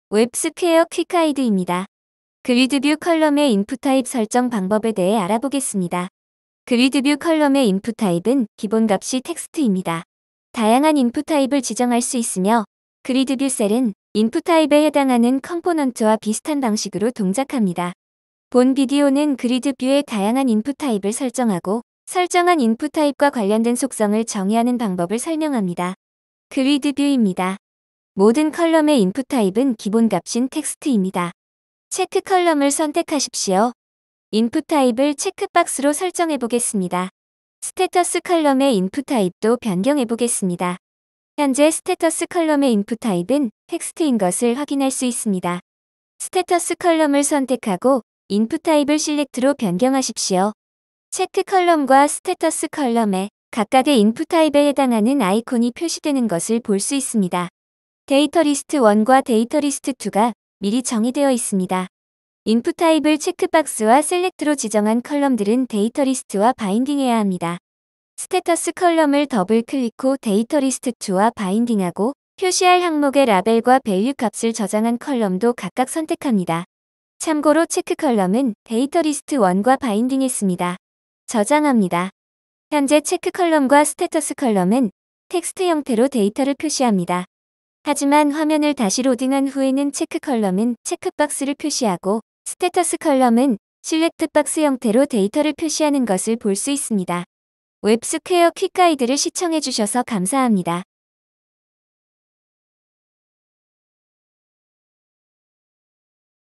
gridView.column - inputType="checkbox" & inputType="select" | WebSquare5 - Quick Guide
00:00 Introduction
00:02 inputType of gridView.column
00:19 How to set inputType in the gridView.column.
00:25 DEMO
01:37 End of DEMO

Set inputType="checkbox" and inputType="select" columns.

Related APIs
   inputType="checkbox"
   inputType="select"